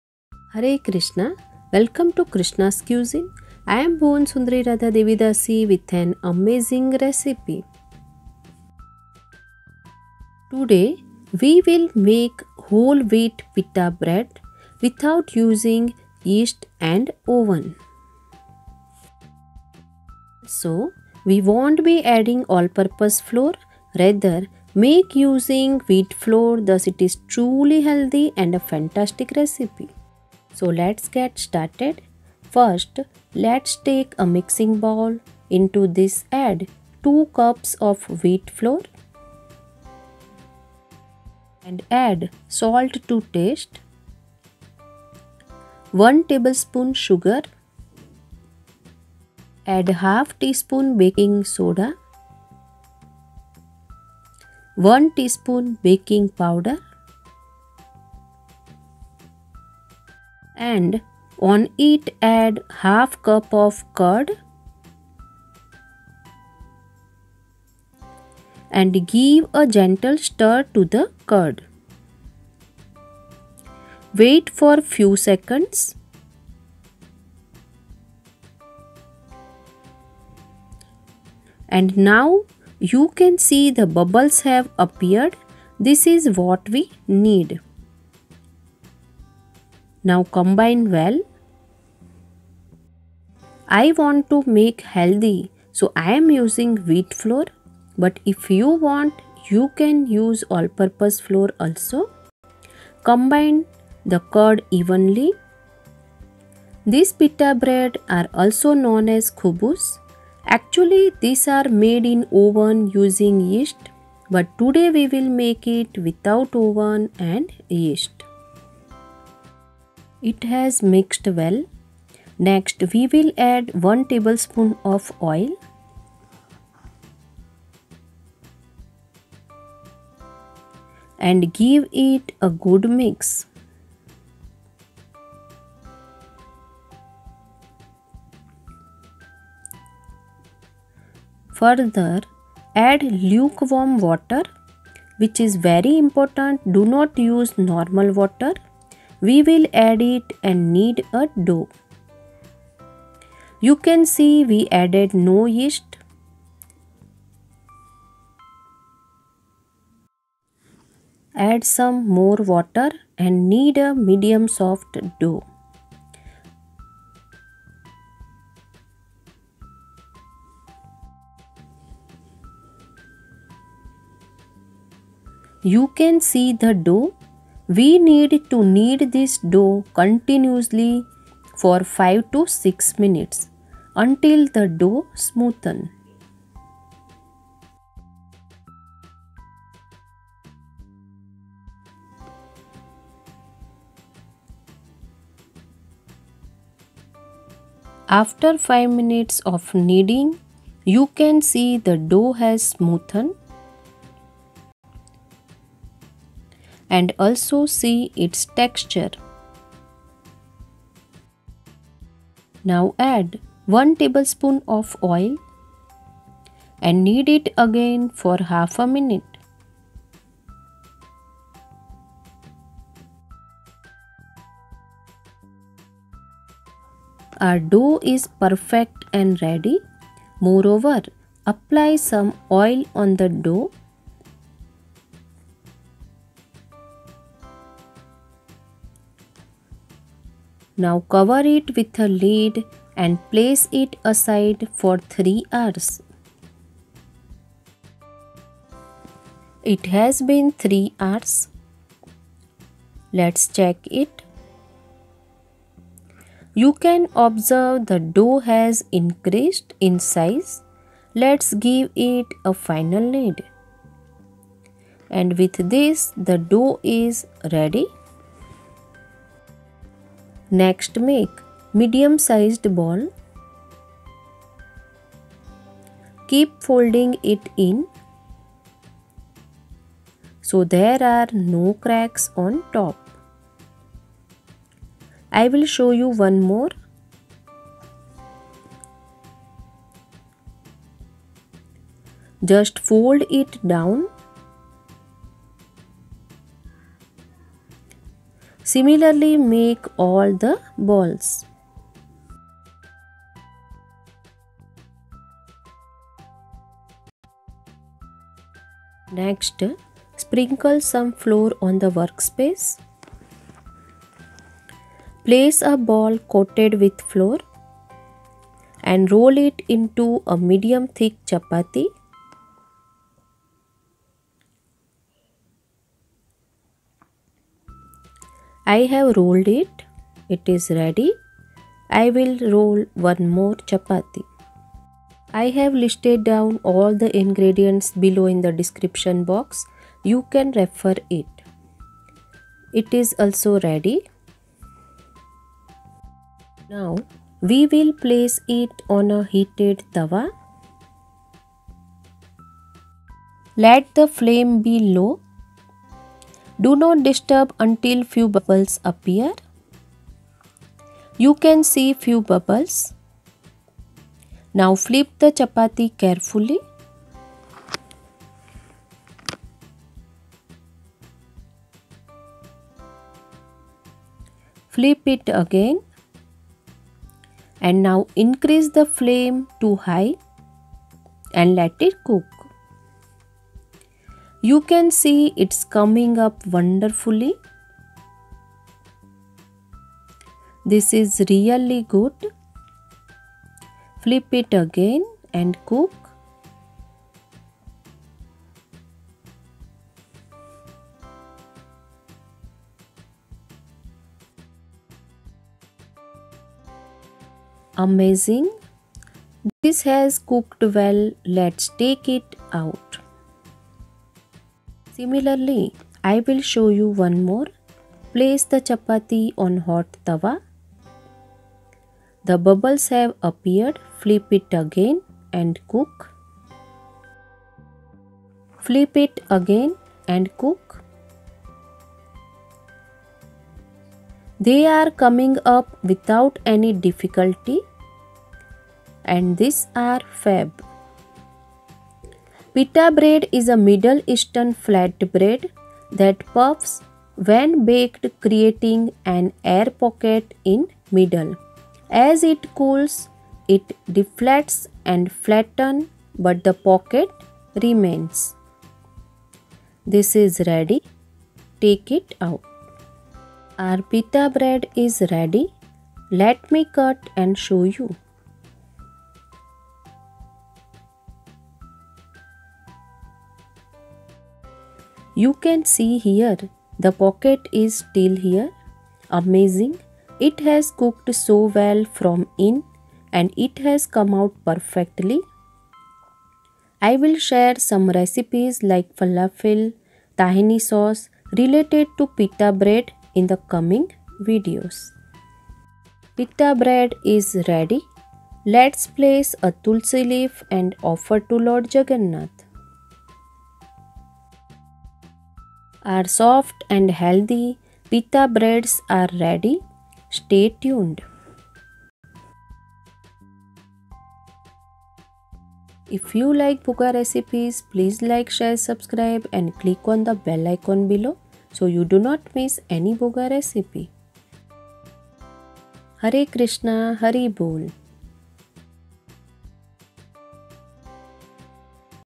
Whole Wheat Pita Bread || Pita Pockets || No Yeast No Oven || Krishna's Cuisine
Hare Krishna!!

Try these soft and healthy pita bread also known as khubus, made up of wholewheat flour without using yeast and oven, and yet get perfect pita breads.
For more recipes, search for Krishna's Cuisine in youtube and scroll down for all the bhoga recipes.

Nos.- 8

Ingredients:

2 cups wheat flour
Salt to taste
1 tbsp sugar
1/2 tsp baking soda
1 tsp baking powder
1/2 cup curd/ yoghurt
1 tbsp oil
Around 1/2 cup lukewarm water
1 tbsp oil